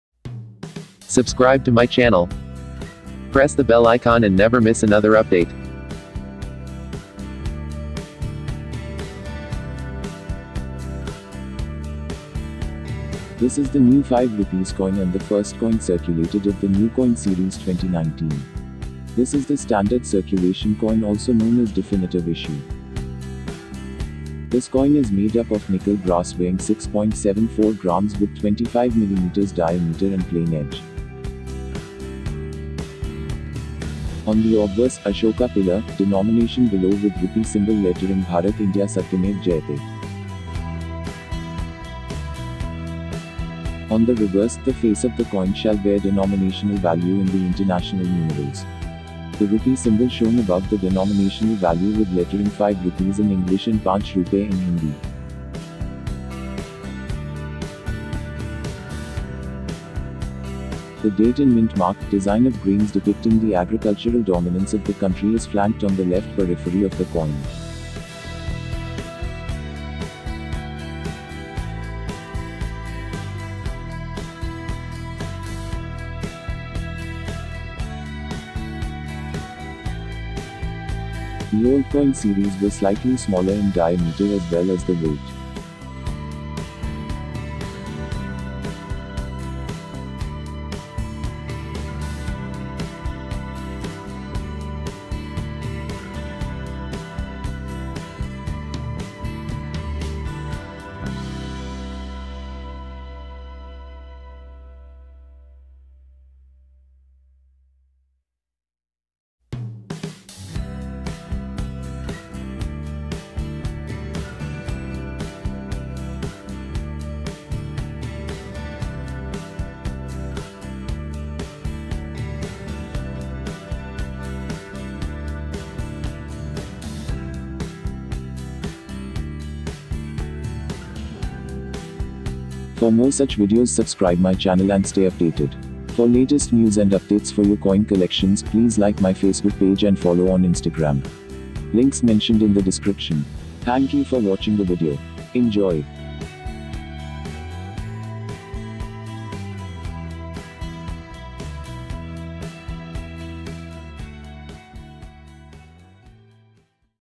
New 5 Rupees Definitive New Coin Series 2019
This is the new 5 Rupees coin and the first coin circulated of the new coin series 2019. This is the standard circulation coin issue or definitive issue.

This coin is made up of Nickel-brass weighing 6.74 grams with 25 mm diameter and plain edge. The old coin series were slightly smaller in diameter as well as the weight.

On the obverse, Ashoka Pillar, Denomination Below with rupee symbol lettering भारत INDIA सत्यमेव जयते.

On the reverse, The face of the coin shall bear denominational value in the international numerals. The Rupee symbol shown above the denominational value with lettering 5 Rupees पांच रूपये. The date and mint mark, design of grains depicting the agricultural dominance of the country is flanked on the left periphery of the coin.